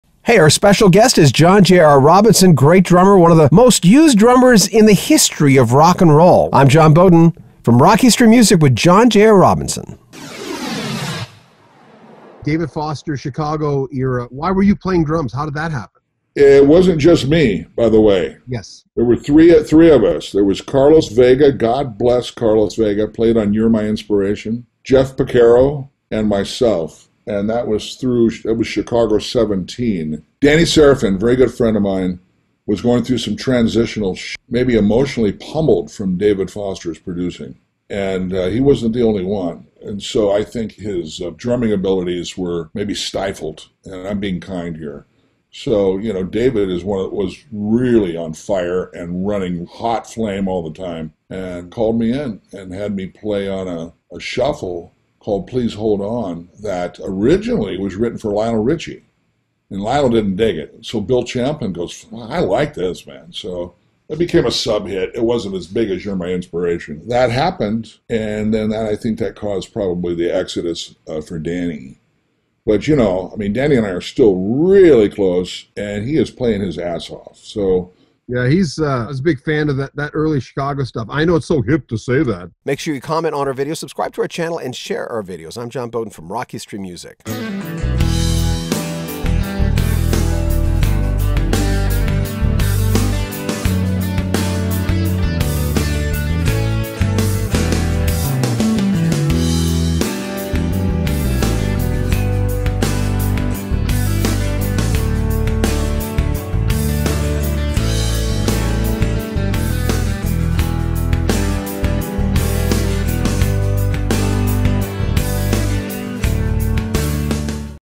INTERVIEW: When John JR Robinson Worked with the band Chicago
Interview CLip #9 INTERVIEW: When John JR Robinson Worked with the band Chicago